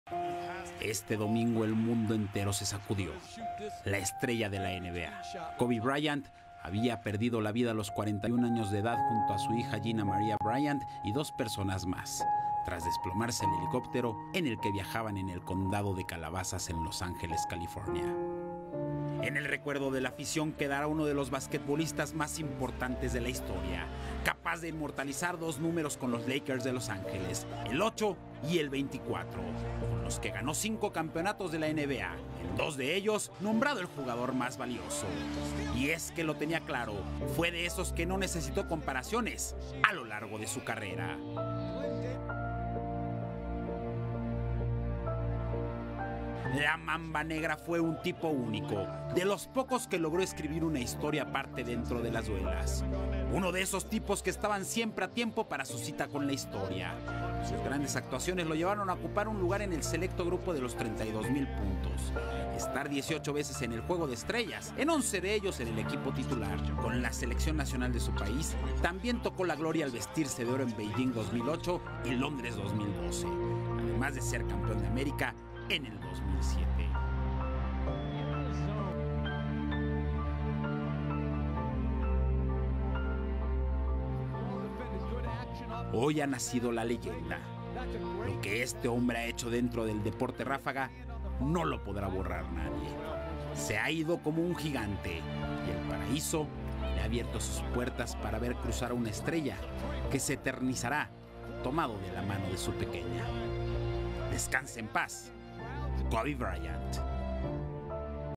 Fallece la estrella de la NBA Kobe Bryant en accidente aéreo a los 41 años de edad .
Hechos AM Puebla. 27 enero 2020.